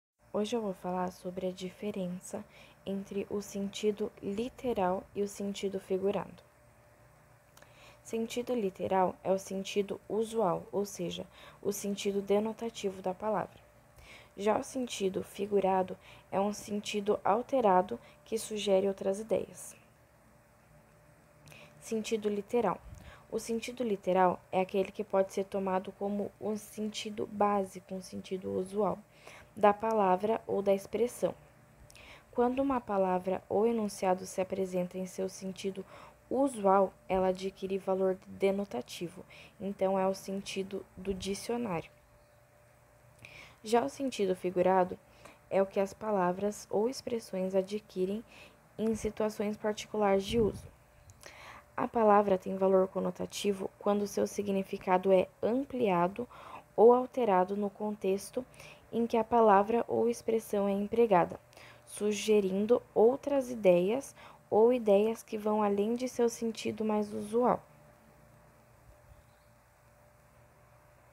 DIFERENÇA ENTRE SENTIDO LITERAL E FIGURADO
Nessa vídeo aula de gramática  mostro a diferença entre o sentido literal (denotativo) e do sentido figurado (conotativo).